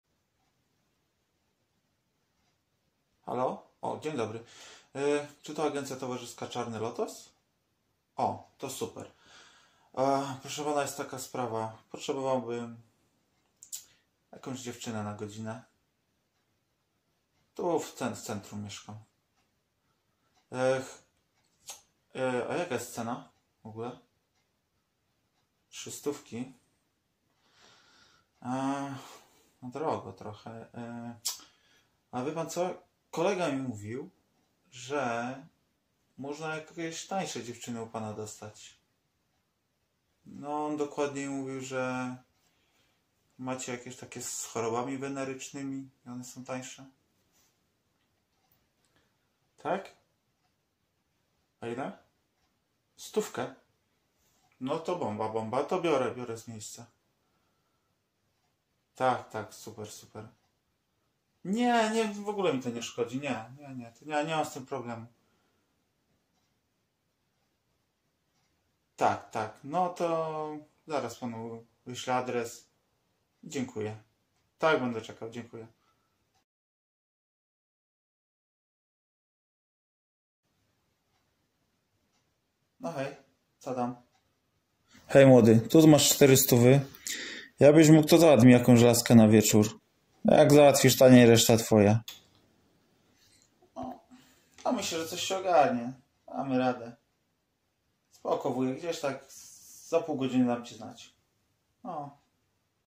Tanie Panie na sponiewieranie - Telefon do burdelu
Telefon do burdelu z prośbą o zniżkę